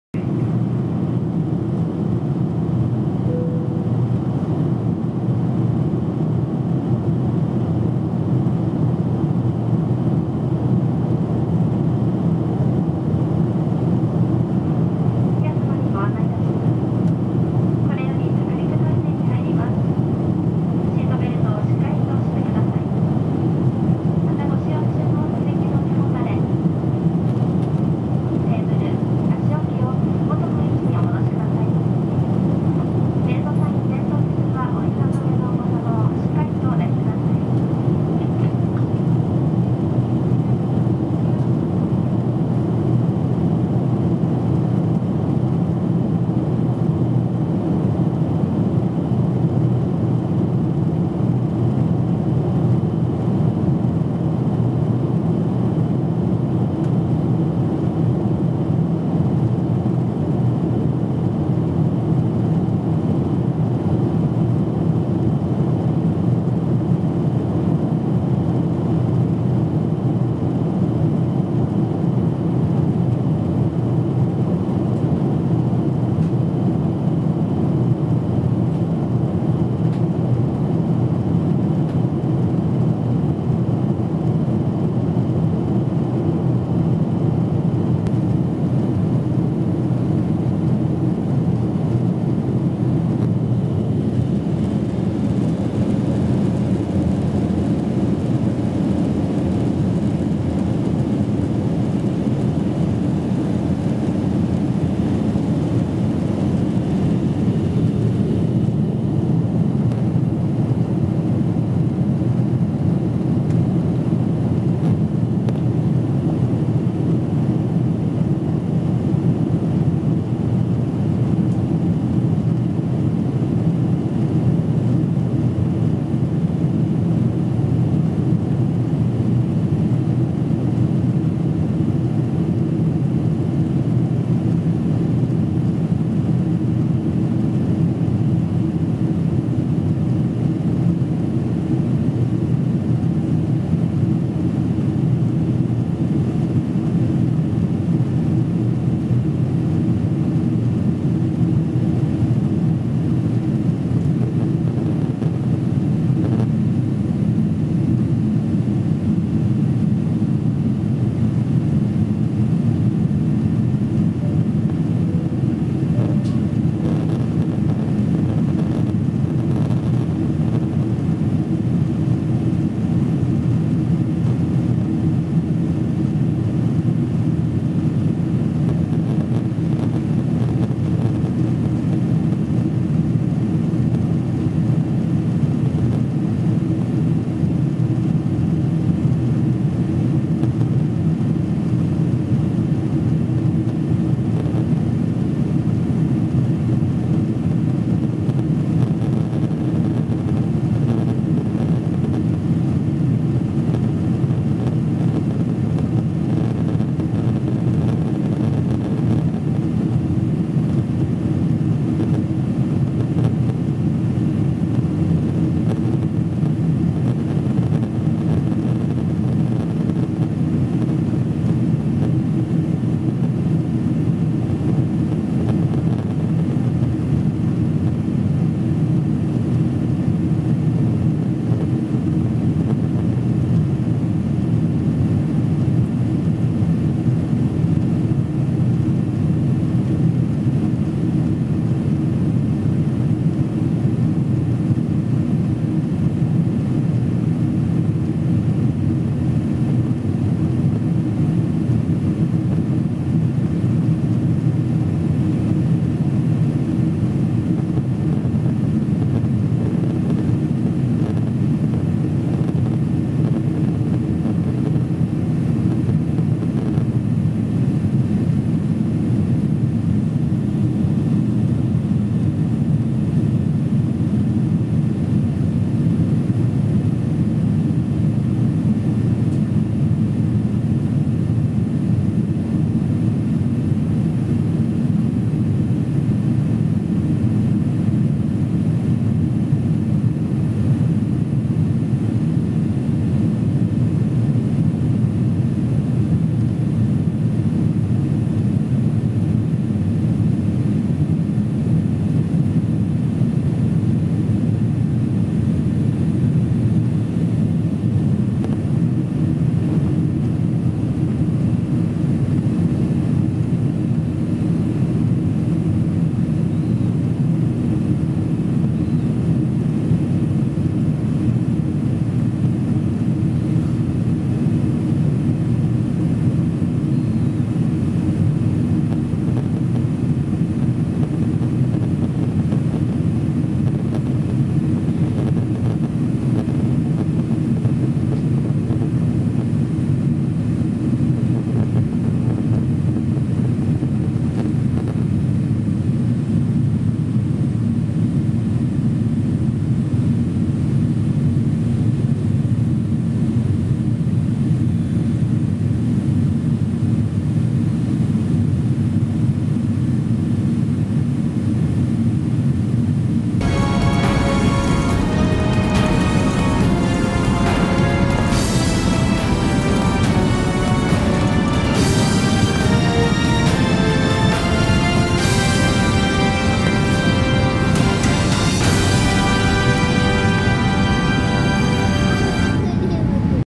房総半島上空／JAL日本航空908便B777-200（旧JAS機）那覇空港→羽田空港
JAL日本航空908便、那覇空港→羽田空港B777-200(JA8979)右側後方座席より撮影した房総半島上空映像をお届けします。
JA8979は旧日本エアシステムの機材です

【撮影機材】
Panasonic 4K VZX990M 64GB HC-VZX990M-W
4K ビデオカメラ VZX992M

Everyday Update!